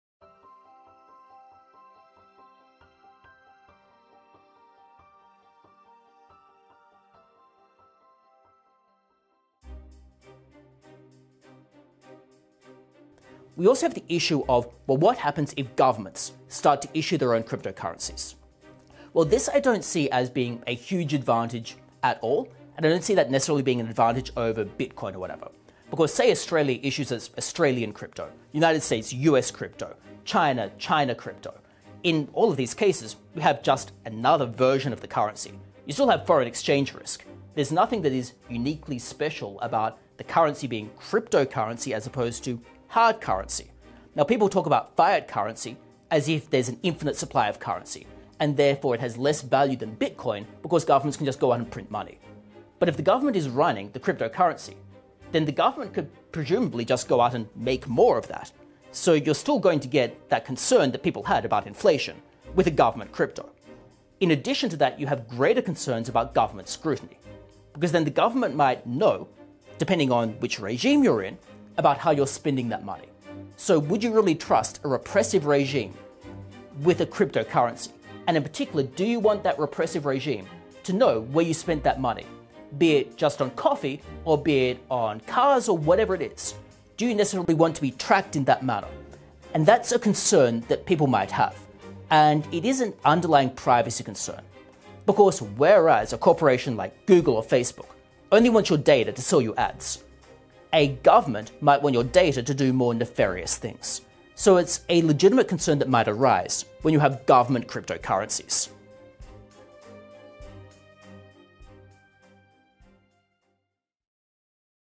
What if governments just create their own cryptocurrency?
Privacy issues and inflation risk are two key concerns, says UNSW Business School’s Mark Humphery-Jenner, AGSM Scholar and Associate Professor in the School of Banking ＆ Finance.